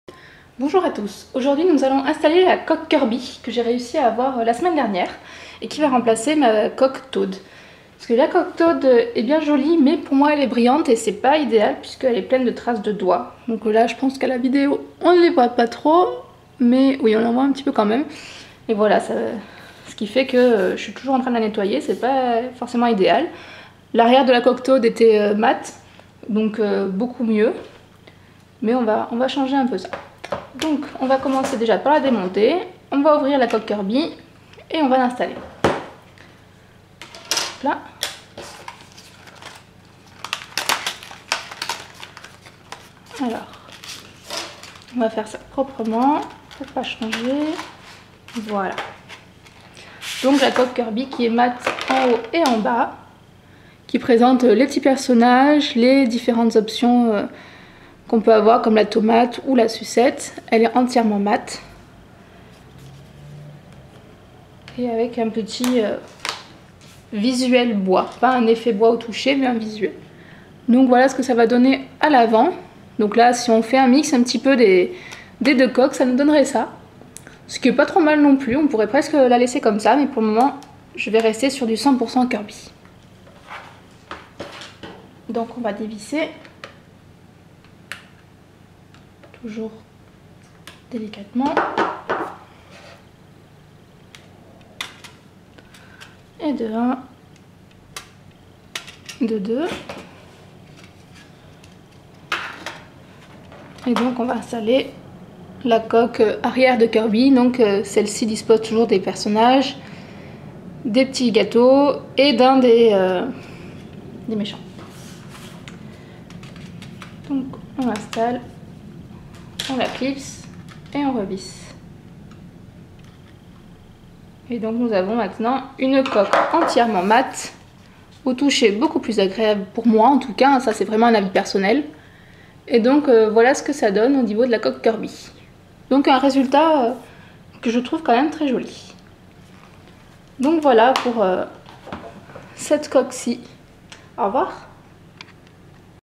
[New3DS] Unboxing coque Kirby
Unoxing de la coque / cover plate Kirby, toute mate à l'effigie de la petite boule rose!

Ӂ ­ Ӂ

☼ Pour me suivre également sur Twitter (nouvelles vidéos, nouveaux articles de mon site, infos diverses sur les jeux 3DS et le monde Nintendo) :

Ӂ ­ Ӂ